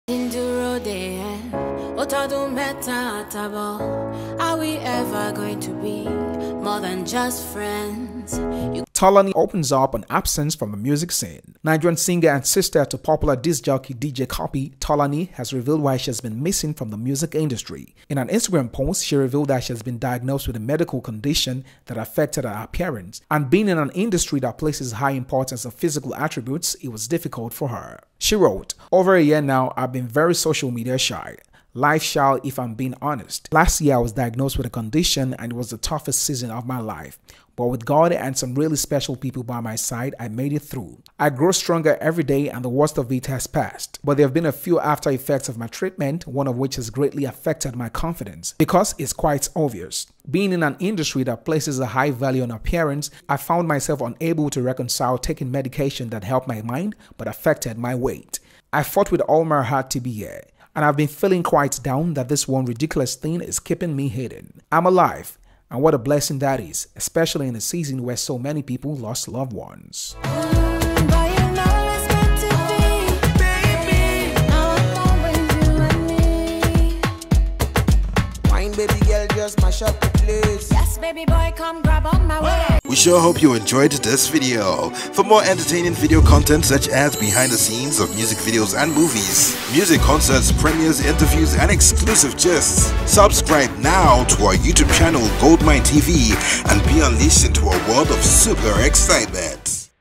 FEMI OTEDOLA’S DAUGHTER TOLANI OPENS UP ON ABSENCE FROM THE MUSIC SCENE
GoldmyneTV brings you the latest events, trending videos, movies, lifestyle, and Nollywood.